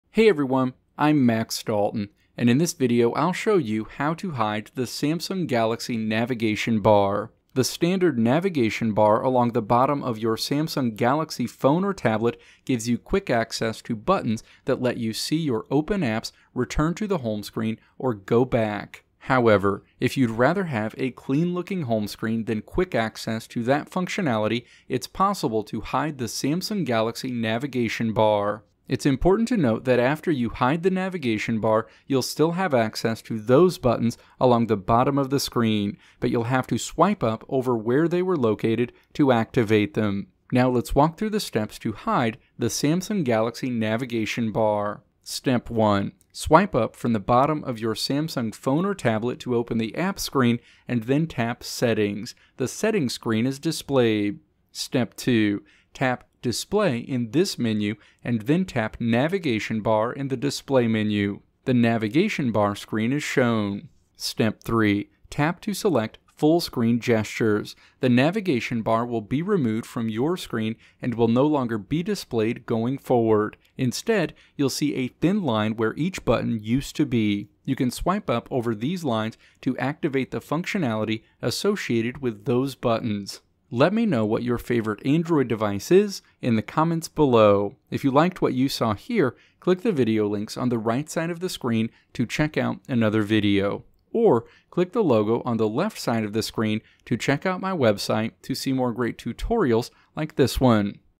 How to Hide Samsung Galaxy Navigation Bar
In this video I'll show you how to hide the Samsung Galaxy navigation bar.

Timestamps:
Introduction: 0:00
Steps to Hide Samsung Galaxy Navigation Bar: 0:40

Video Transcript:
The standard navigation bar along the bottom of your Samsung Galaxy phone or tablet gives you quick access to buttons that let you see your open apps, return to the home screen, or go back. However, if you'd rather have a clean looking screen than quick access to that functionality, it's possible to hide the Samsung Galaxy navigation bar. It's important to note that after you hide the navigation bar you'll still have access to those buttons along the bottom of the screen, but you'll have to swipe up over where they were located to activate them.

Now let's walk through the steps to hide the Samsung Galaxy navigation bar.

Step 1. Swipe up from the bottom of your Samsung phone or tablet to open the Apps screen, and then tap "Settings." The Settings screen is displayed.

Step 2. Tap "Display" in this menu, and then tap "Navigation Bar" in the Display menu. The Navigation Bar screen is shown.

Step 3. Tap to select "Full Screen Gestures." The navigation bar will be removed from your screen, and will no longer be displayed going forward. Instead you'll see a thin line where each button used to be. You can swipe up over these lines to activate the functionality associated with those buttons.